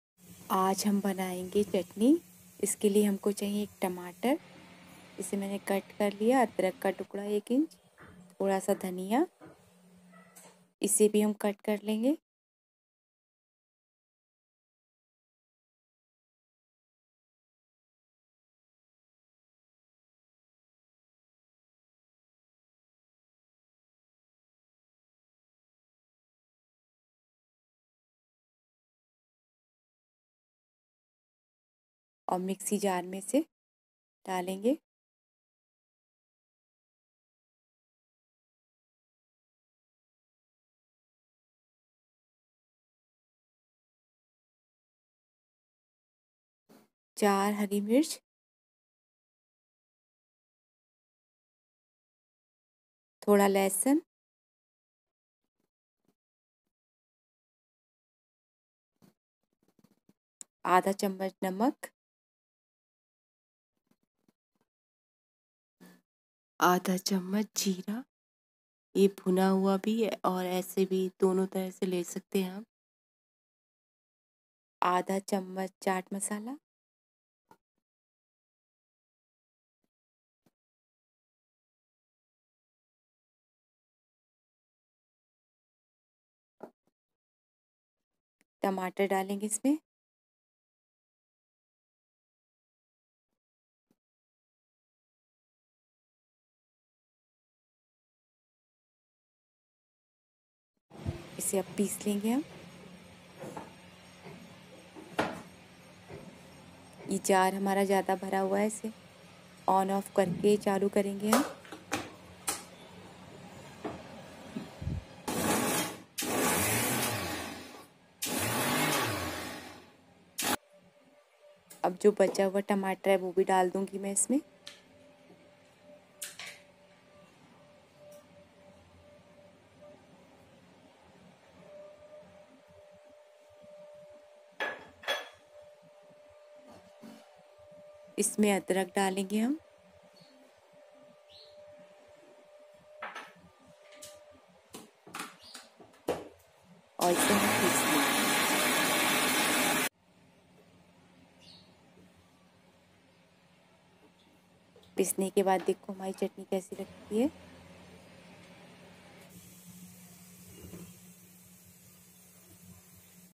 हरी धनिया की चटपटी चटनी बनाओगे तो उंगली चाटते रह जाओगे ll Desi chutney recipe
# green chutney

Dhaniya lahsun ki Chatni
Kaise banaen